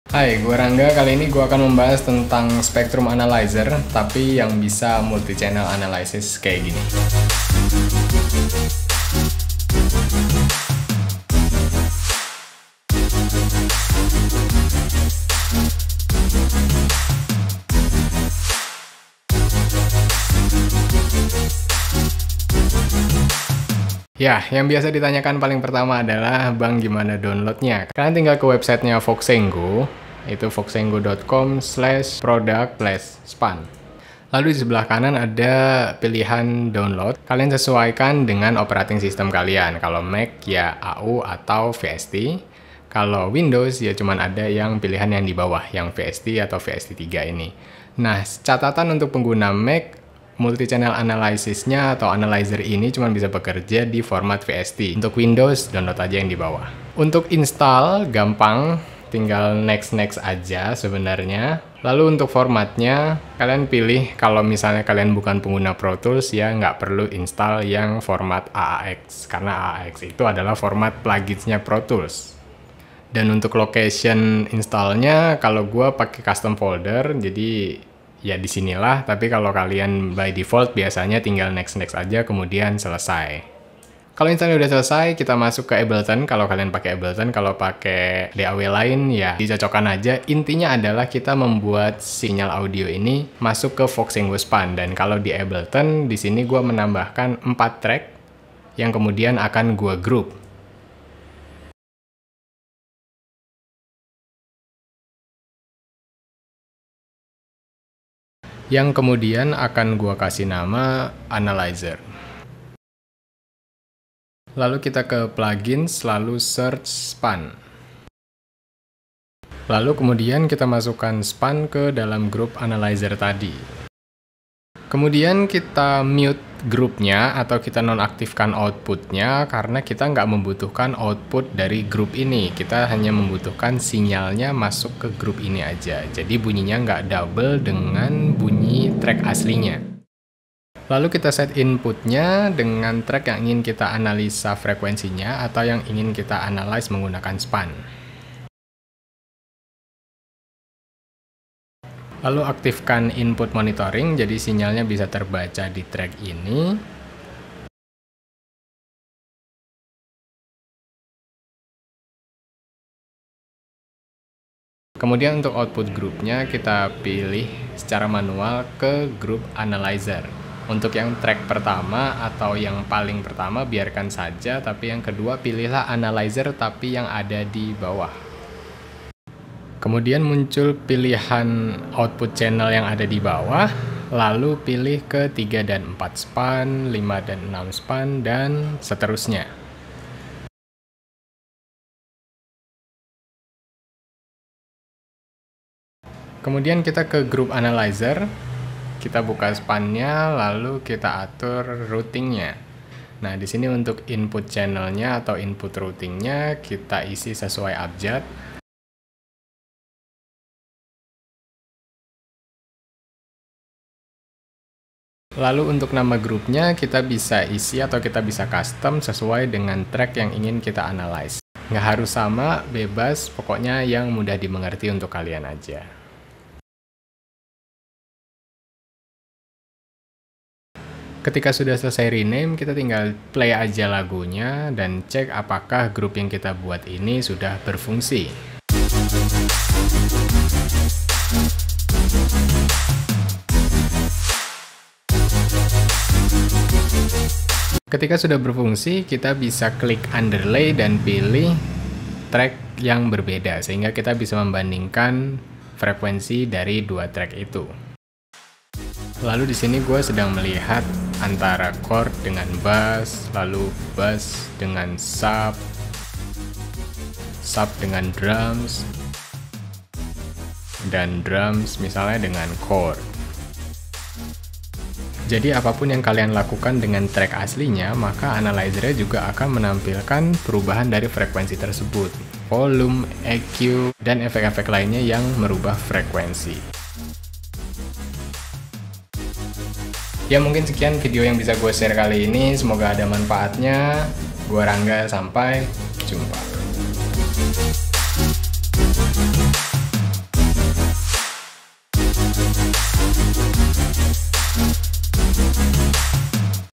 Plugins Untuk Mixing | Agar Mixing jadi Lebih Mudah
Bukan Tutorial Caranya Mixing yaa ~

Disini saya akan membahas Plugins, yang bisa kita gunakan untuk membantu mixing frequency, karena tidak semua orang memiliki telinga dengan kemampuan "Audio Engineering" yang baik, maka tidak ada salahnya kita menggunakan tampilan visual untuk membantu kita mixing dan menemukan frequency yang clash atau masih nabrak.